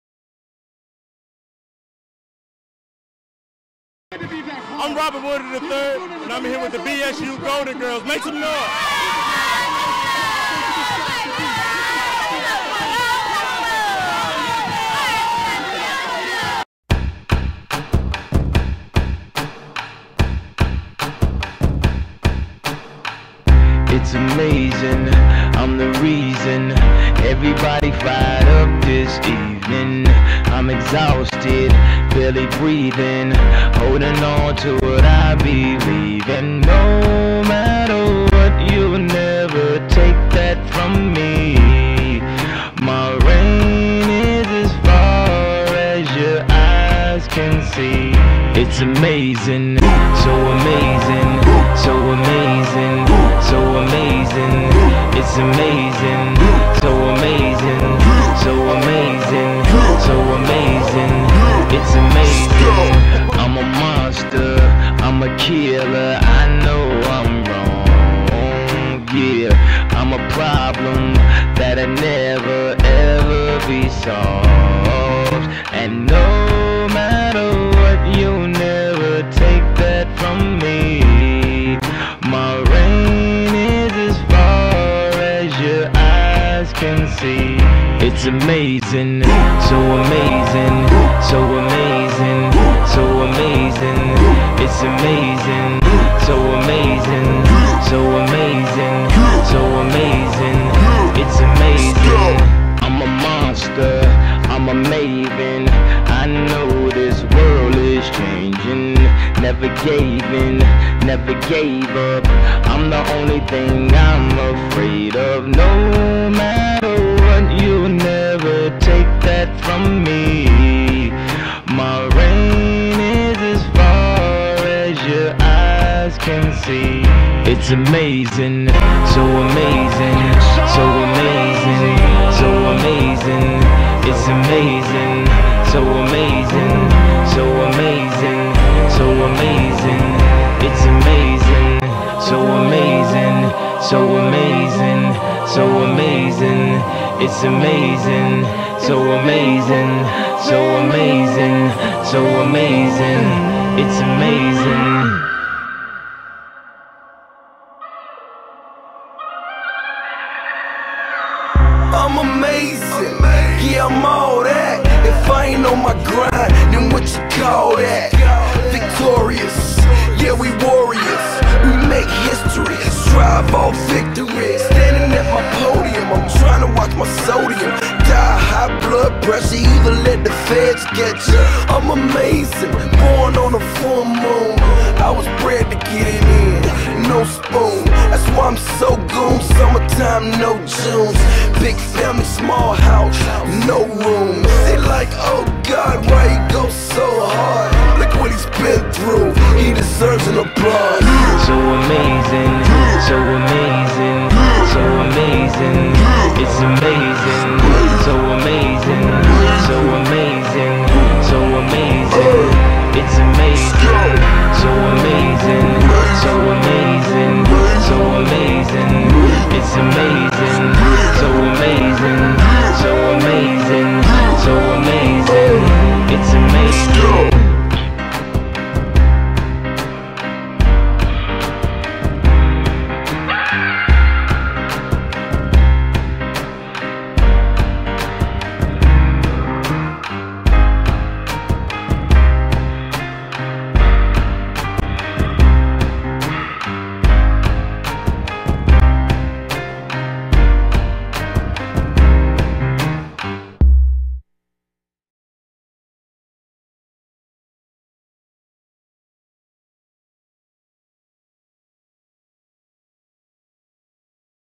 BSU Athletics Program 2008-2009
Highlight tape of the Bowie State University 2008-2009 Athletics Program
Edited by Sheldon Boodie